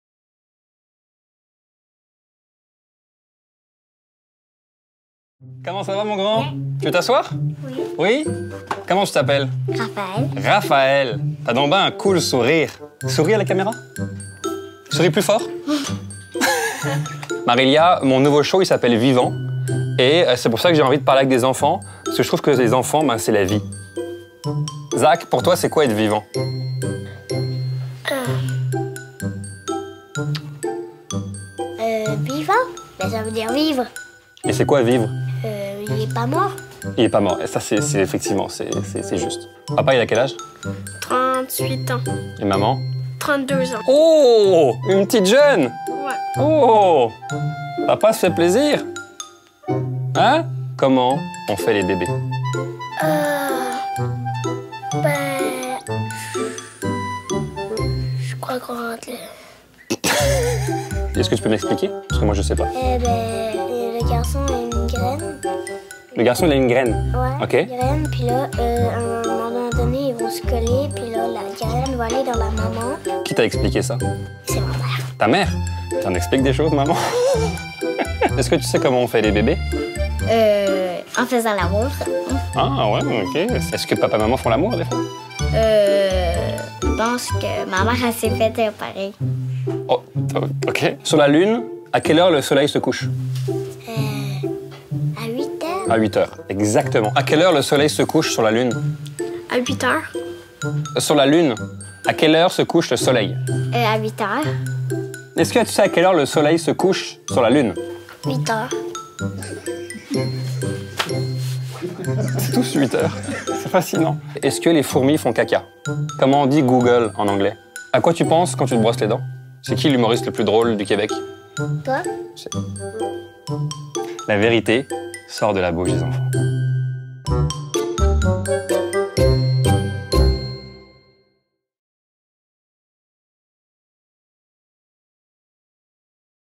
Jérémy Demay | Théâtre des Deux Rives | 20 janvier 2018 à 20h00
Le spectacle de Jérémy Demay sera présenté au Cabaret-Théâtre du Vieux-Saint-Jean le 20 janvier 2018 à 20 h.

Information et billetterie ou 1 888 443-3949